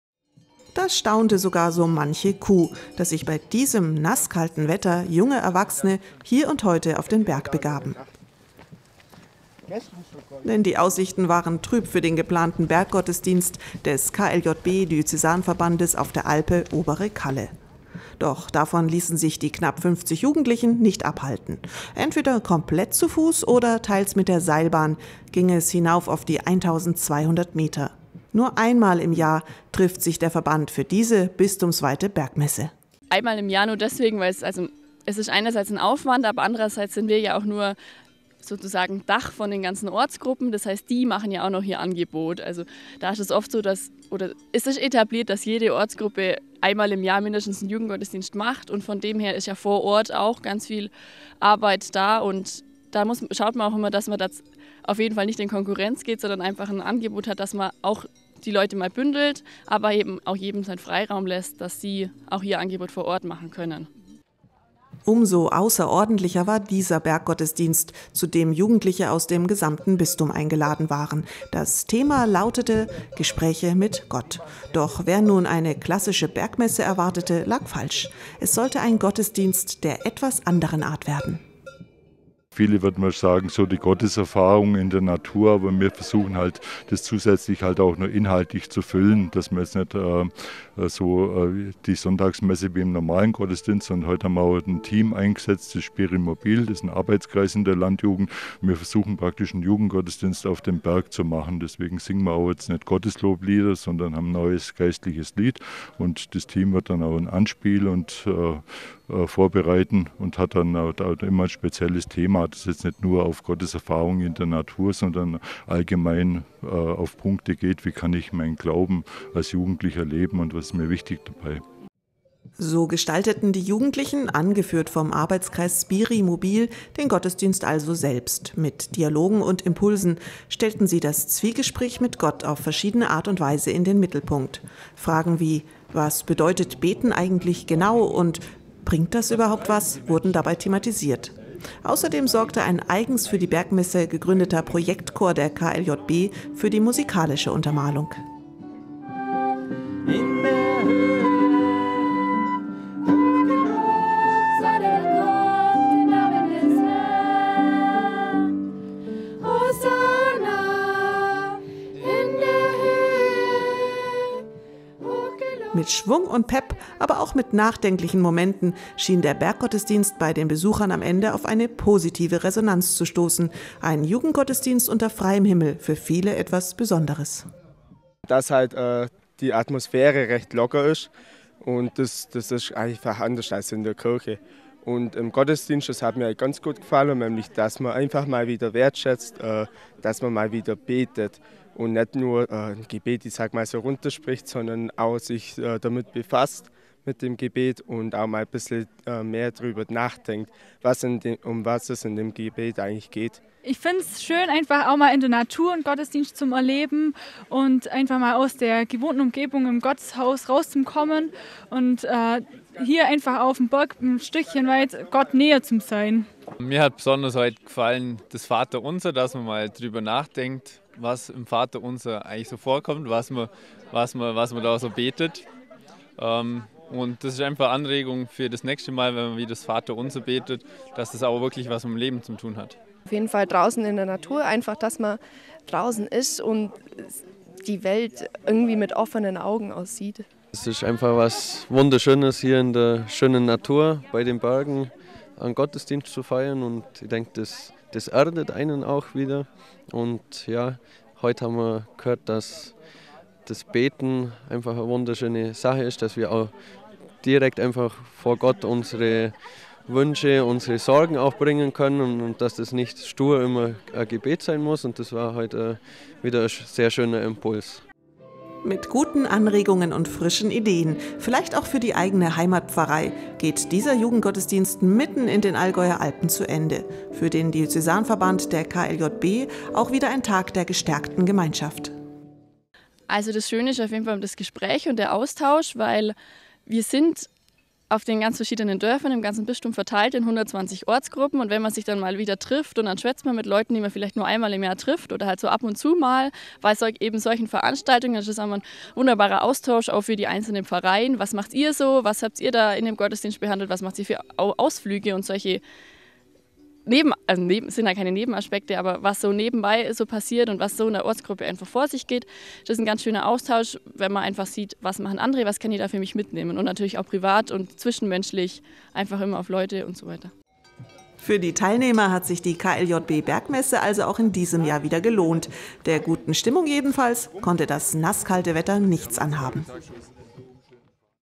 Katholische Landjugend feiert Bergmesse
Die Katholische Landjugendbewegung KLJB vertritt seit 1947 die Interessen junger Menschen in ländlichen Räumen und engagiert sich für eine aktive und lebendige Kirche. Innerhalb der Diözese Augsburg hat der Jugendverband im Moment rund 3700 Mitglieder. Und da viele von ihnen im Allgäu wohnen, liegt es nahe, dass die schwäbische KLJB auch Bergmessen veranstaltet. Wie schon in den Jahren zuvor trafen sich die Teilnehmer nun wieder an der Alpsee Bergwelt bei Immenstadt, um von dort aus zu starten.